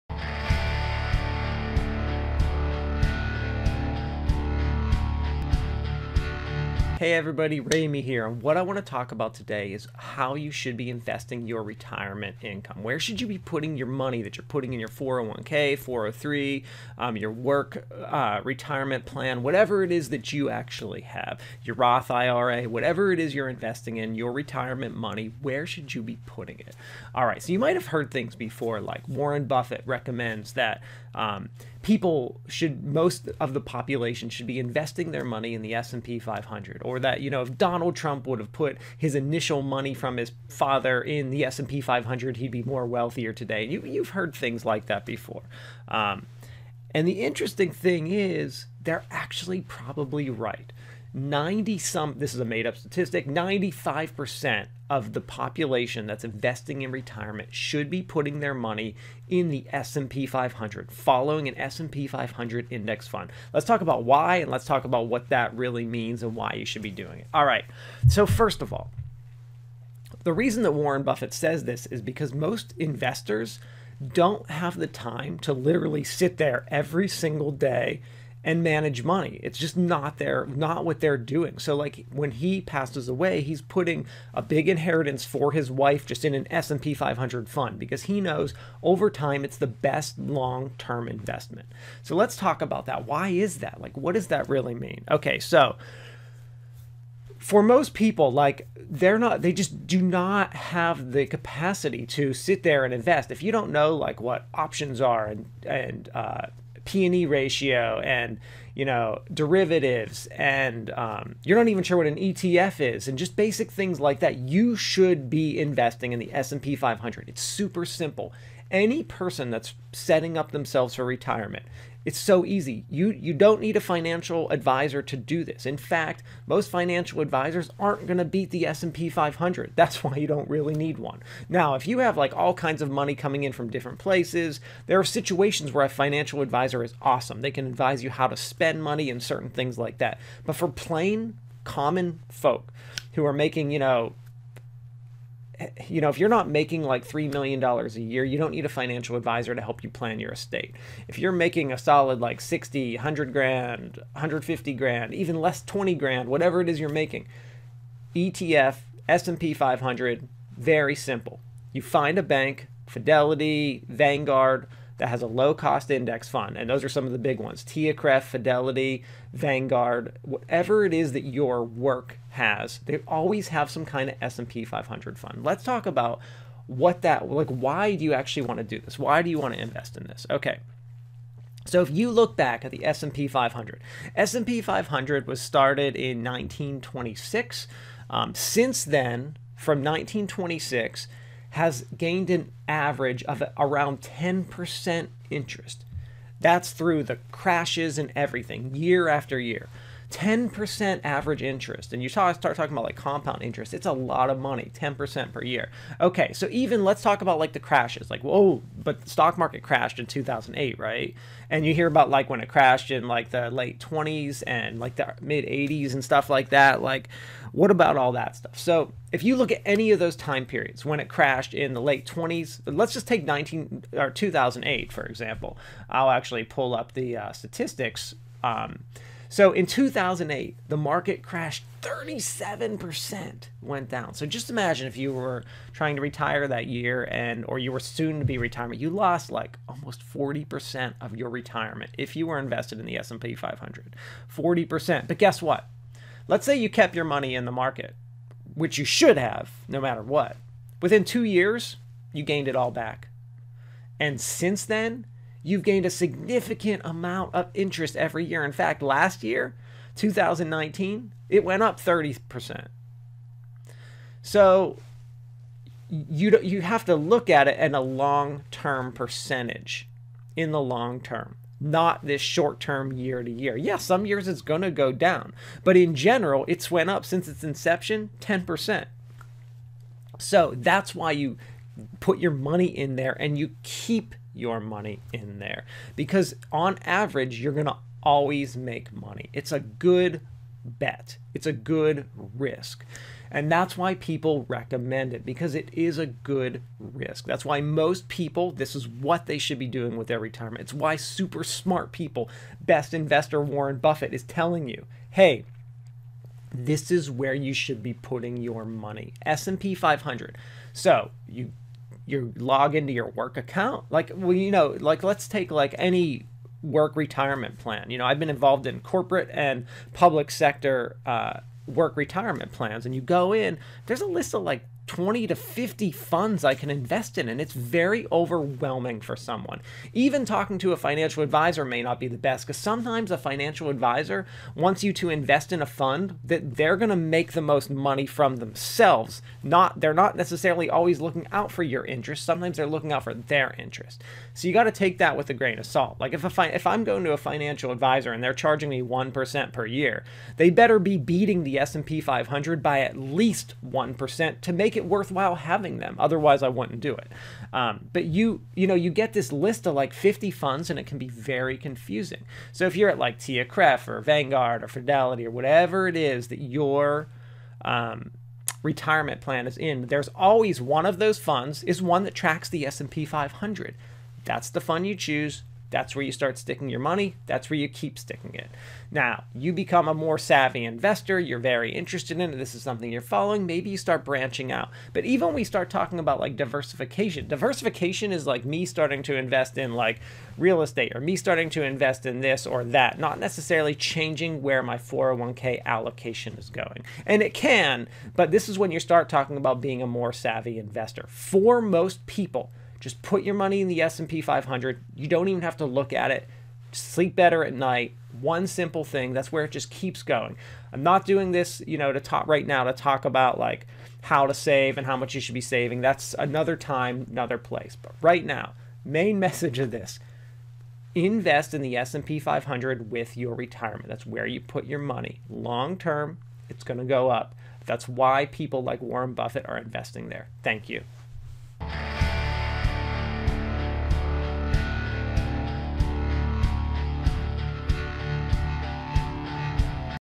Where to invest your 401k or retirement accounts
This video discusses how you should invest your money that you are saving for retirement. It specifically talks about why investing in the S&P 500 is the best way to passively invest for most of the population. ____________________________________________